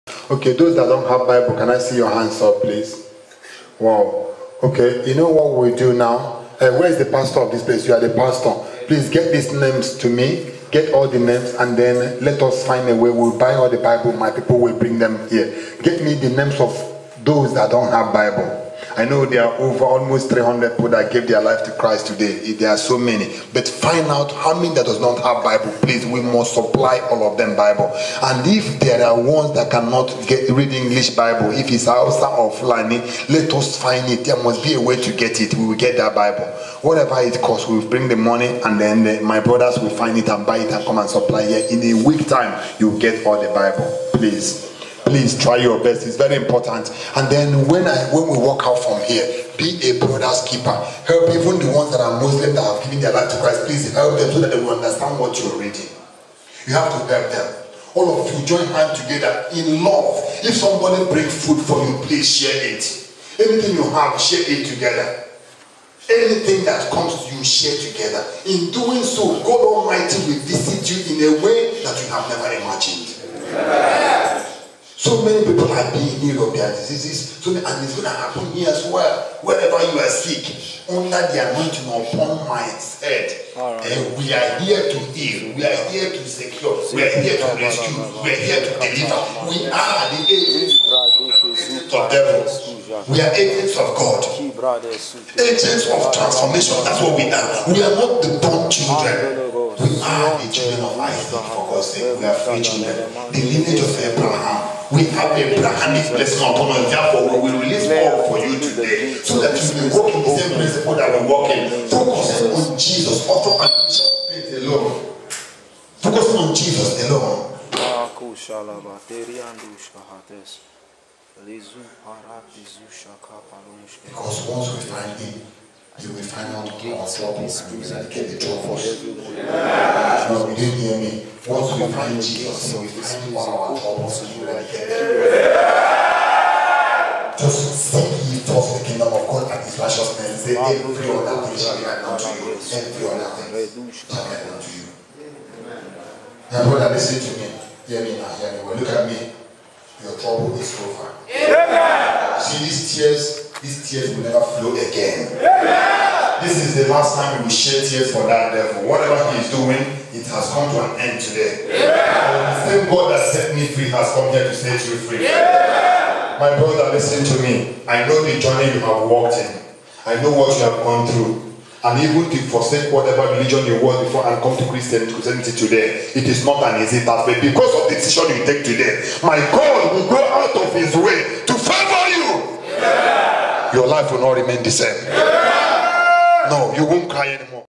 90 percent Auchi prison while is Muslim have surrendered to Jesus today and the figure and statistic
It's a video you can't regret watching.